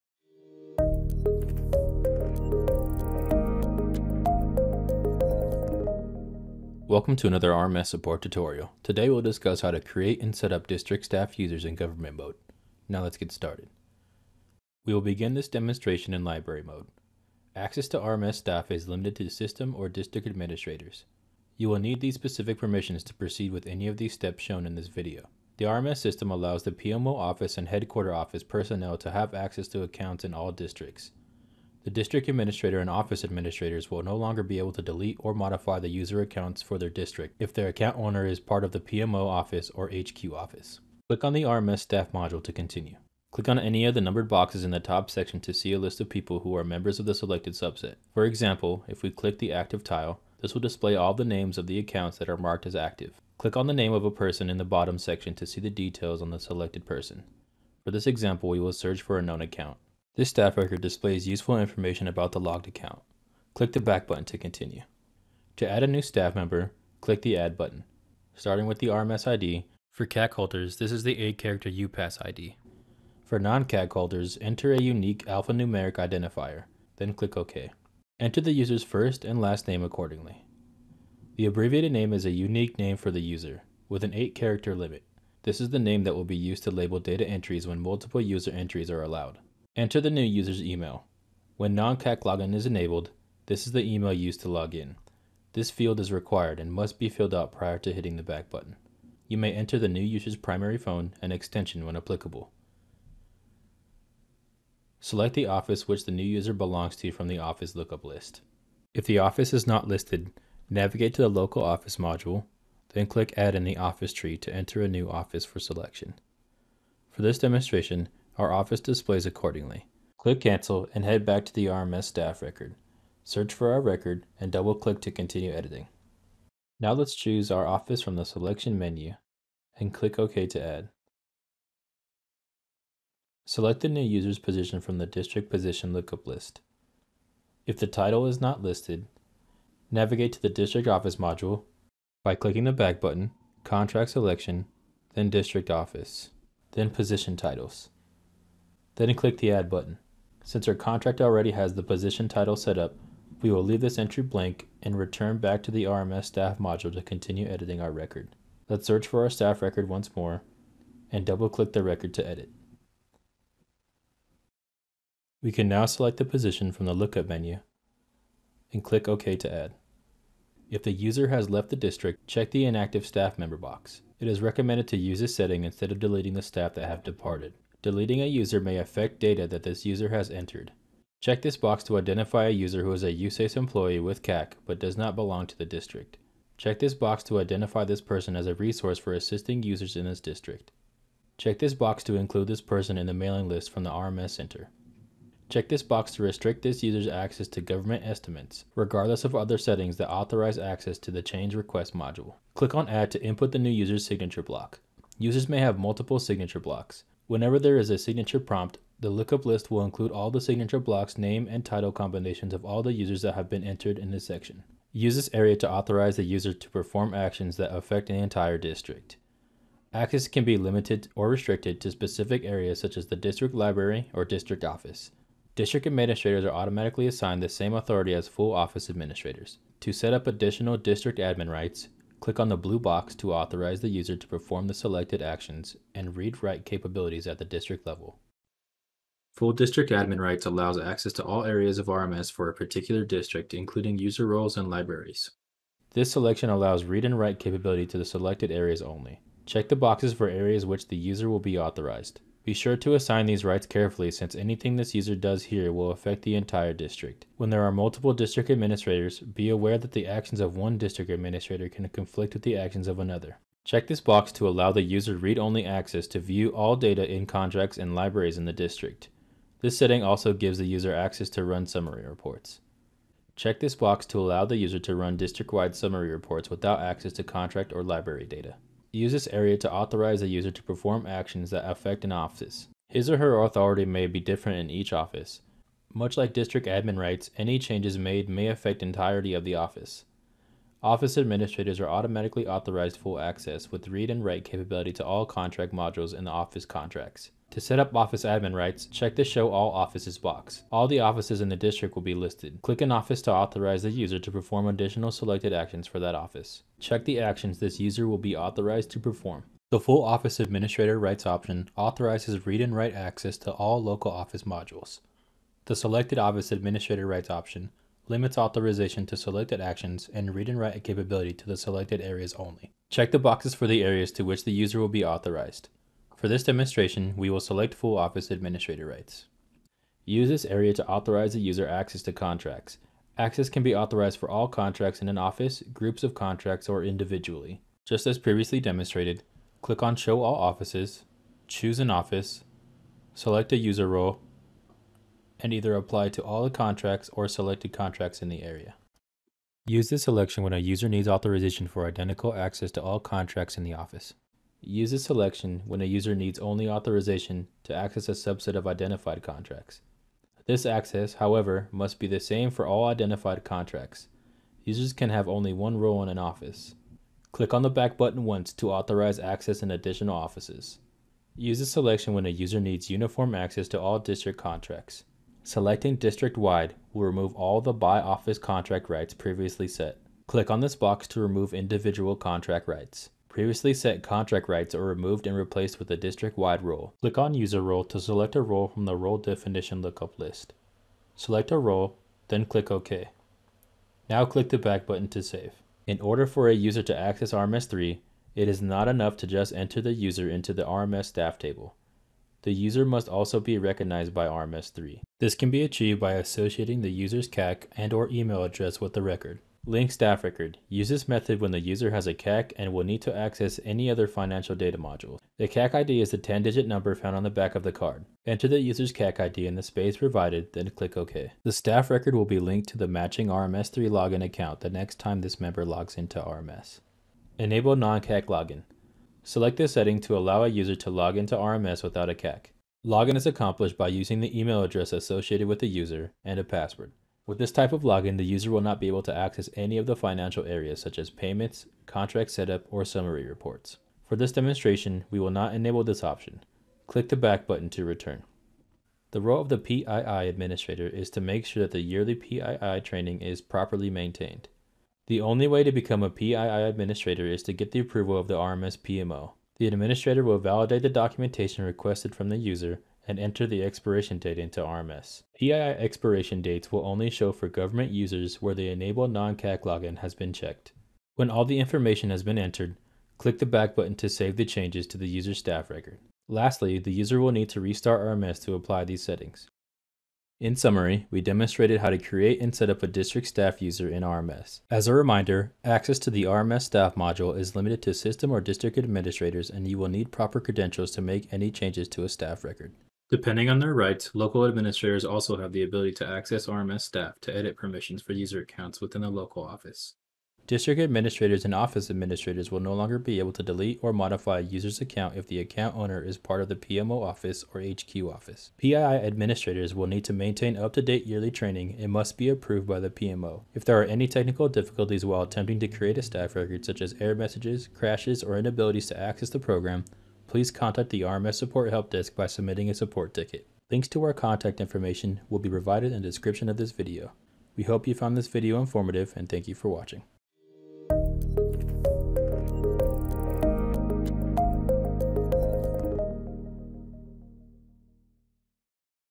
How to Create and Setup District Staff Users (GOV)
Table of Contents:

00:07 - Introduction
00:14 - Creating and Setting Up District Staff Users
01:05 - Adding User
03:22 - Adding Signature Blocks
03:50 - Adding District Admin Rights
04:39 - Adding Office Admin Rights
05:30 - Adding Contract Rights
06:49 - Linking Staff Record
07:08 - Enabling Non CAC Login
08:00 - Summary

If assistance is needed by phone, please call our Support Desk at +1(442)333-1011